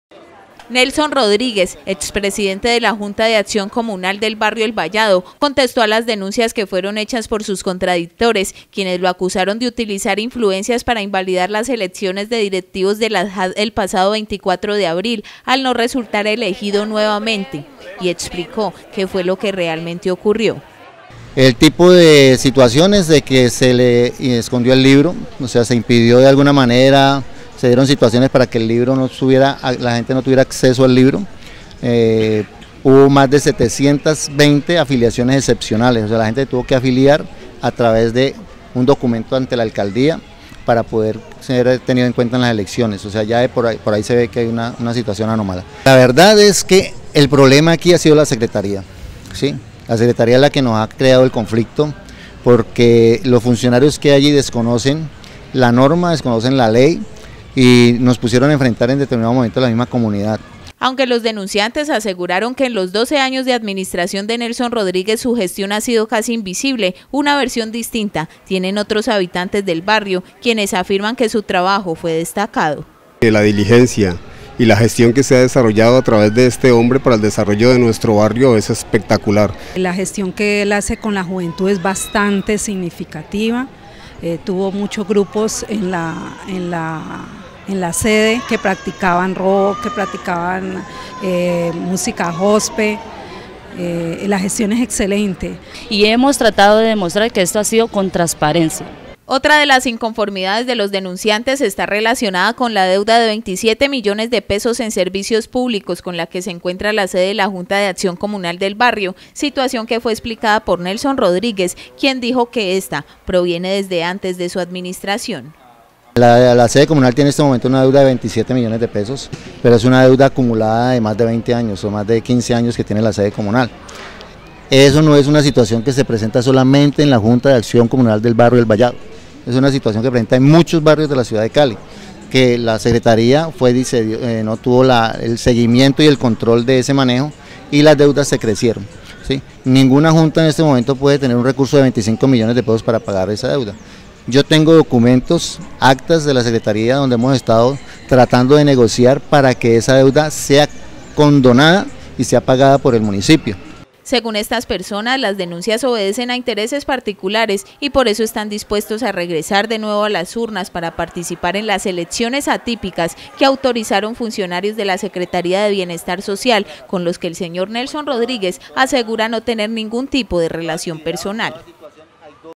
Expresidente de JAC de El Vallado responde a sus críticos y dice que su gestión fue transparente
Nelson Rodríguez, expresidente de la Junta de Acción Comunal del barrió El Vallado, contestó a las denuncias que fueron hechas por sus contradictores, quienes lo acusaron de utilizar influencias para invalidar las elecciones de directivos de las JAC el pasado 24 de abril, al no resultar elegido nuevamente, y explicó que fue lo que realmente ocurrió.

Aunque los denunciantes aseguraron que en los doce años de administración de Nelson Rodríguez su gestión ha sido casi invisible, una versión distinta tienen otros habitantes del barrio, quienes afirman que su trabajo fue destacado.

Otra de las inconformidades de los denunciantes está relacionada con la deuda de 27 millones de pesos en servicios públicos con la que se encuentra la sede de la Junta de Acción comunal del barrio, situación que fue explicada por Nelson Rodríguez, quien dijo que la deuda proviene desde antes de su administración.

Según estas personas, las denuncias obedecen a intereses particulares y por eso están dispuestos a regresar de nuevo a las urnas para participar en las elecciones atípicas que autorizaron funcionarios de la Secretaría de Bienestar Social con los que el señor Nelson Rodríguez asegura no tener ningún tipo de relación personal.

Por: Natalia Vinasco